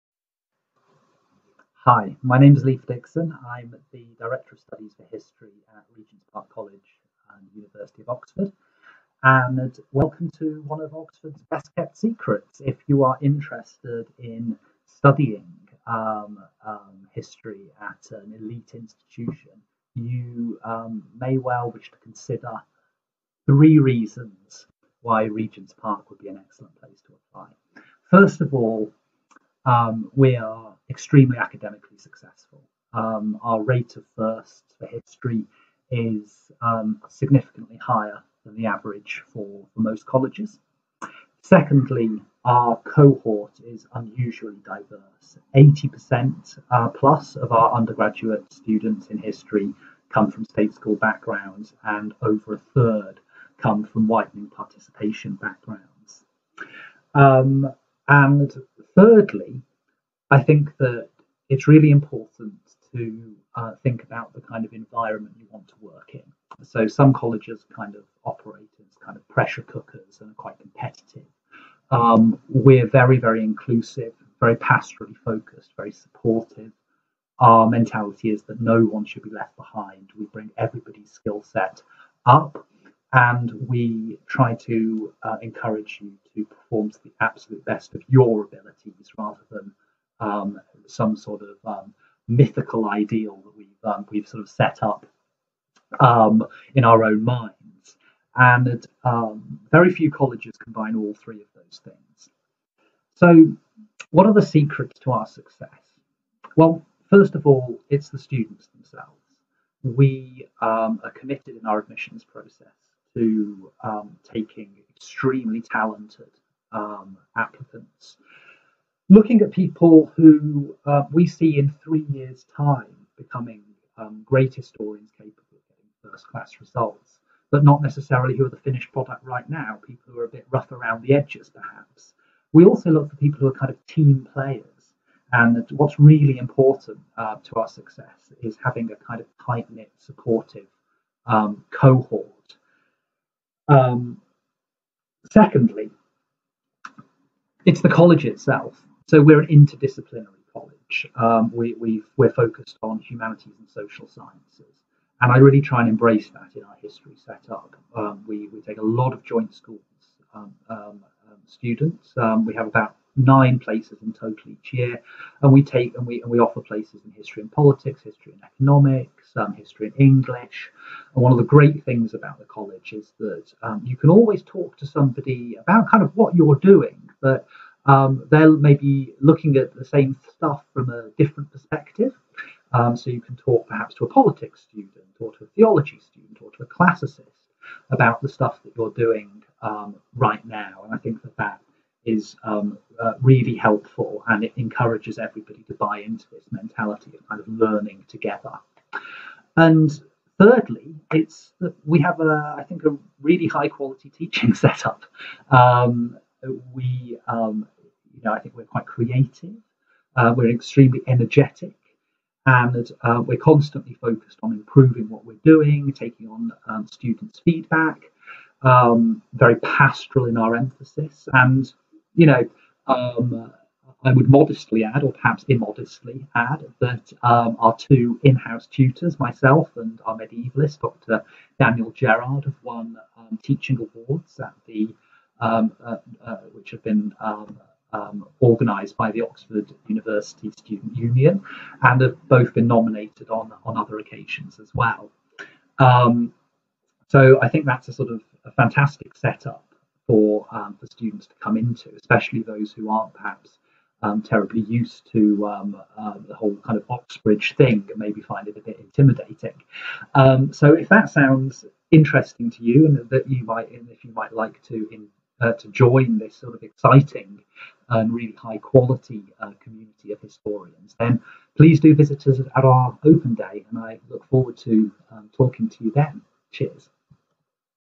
History at Regent’s Park College - September 2020 Open Day
Director of Studies in History, Dr Leif Dixon, discusses History at Regent’s.